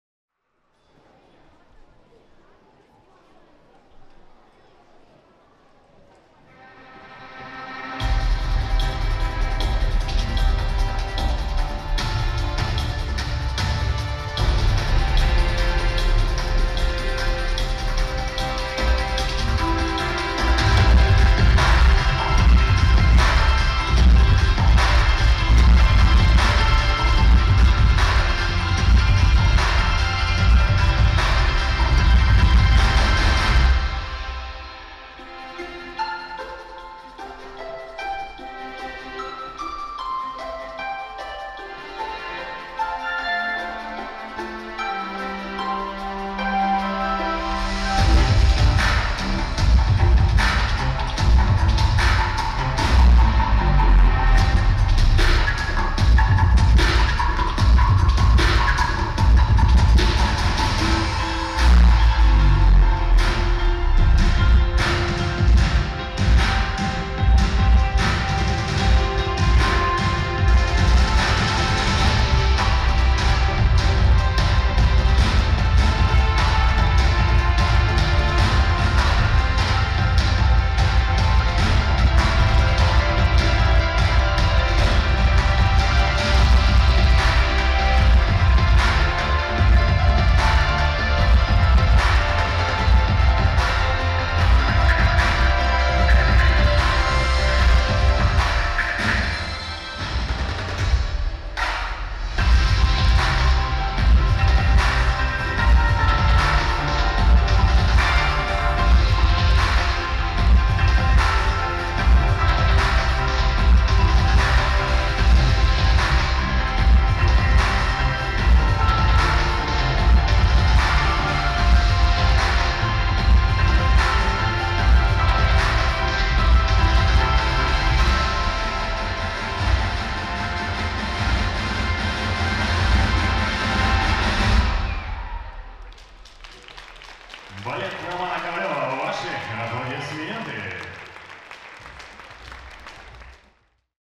Балет Романа Ковалёва / Открытие вечернего отделения Capital Cup Minsk 2021 (Минск, 16.10.2021)
Выступления балета Романа Ковалёва на  открытии вечернего отделения  соревнований по спортивным бальным танцам Capital Cup Minsk 2021 (Минск, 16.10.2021)